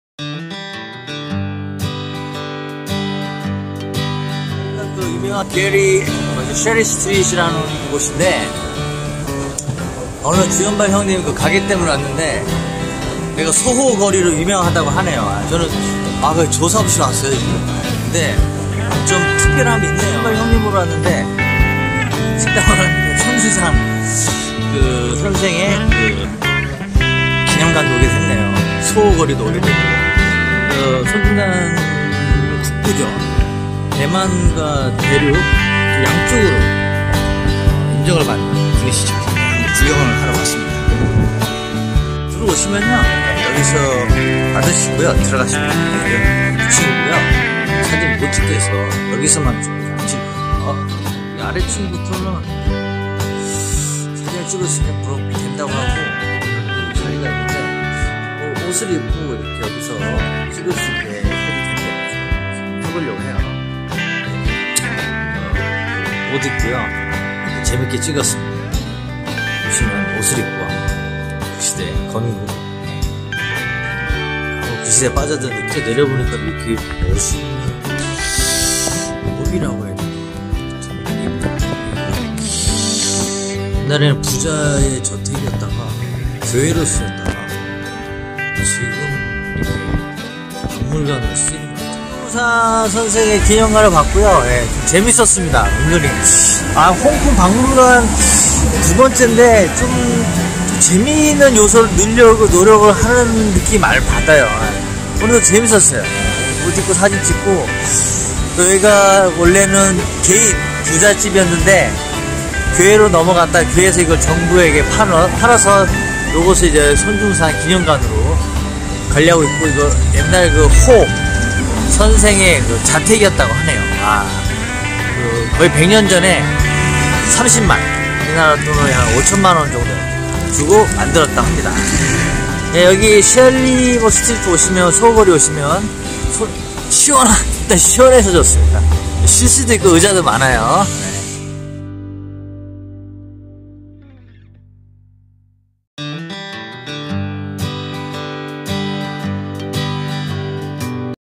홍콩 박물관 손중산 기념관 가보기, 홍콩을 좀더 깊게 보기
홍콩 여행 코스 중에 박물관을 권합니다. 시원하고, 홍콩을 좀더 깊게 볼 수 있는 기회를 줍니다.주윤발 맛집과 소호 거리 근처에 있는 손중산 기념관 정보 입니다.